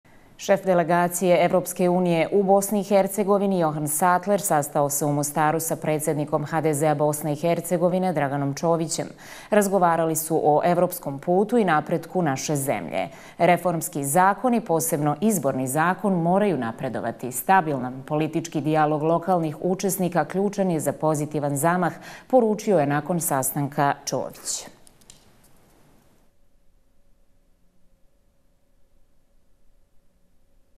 Čović i Sattler u Mostaru: Reformski zakoni, posebno Izborni zakon, moraju napredovati!
Čović i Sattler u Mostaru: Reformski zakoni, posebno Izborni zakon, moraju
napredovati!

Marketing, YouTube & Digital distribution:
EMDC Network